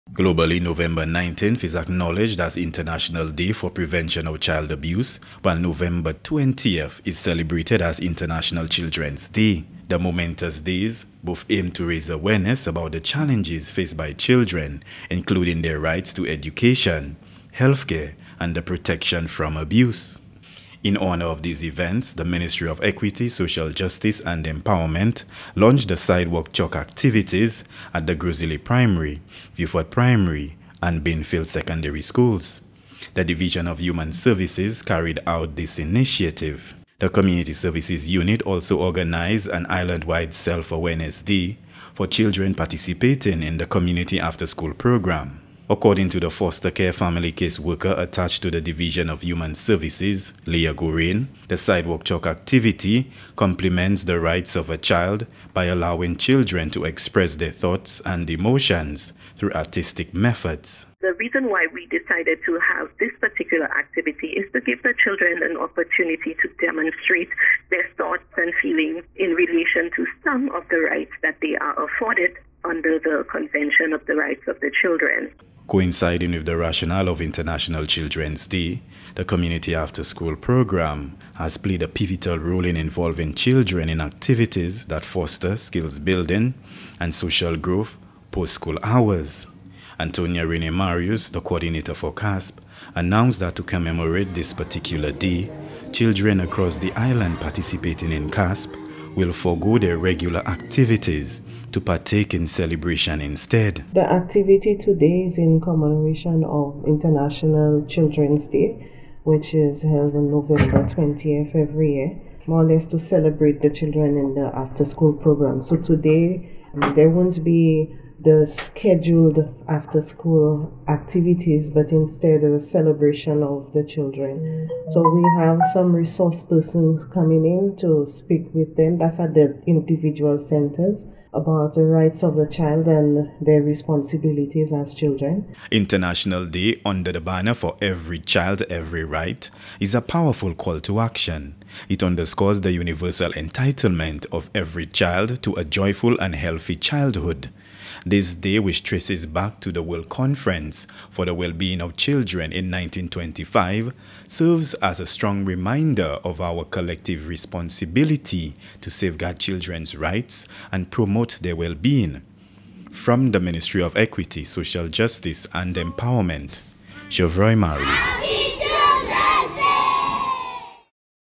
International Children’s Day & the Prevention of Child Abuse Commemorated in Saint Lucia
Globally, November 19 is acknowledged as the International Day for the Prevention of Child Abuse, while November 20 is celebrated as International Children’s Day. The momentous days both aim to raise awareness about the challenges faced by children, including their rights to education, healthcare, and protection from abuse. In honor of these events, the Ministry of Equity, Social Justice, and Empowerment launched the Sidewalk Chalk Activities at the Gros Islet Primary, Vieux Fort Primary, and Beanfield Secondary Schools. The Division of Human Services carried out this initiative. The Community Services Unit also organized an island-wide Self-Awareness Day for children participating in the Community After-School Program (CASP).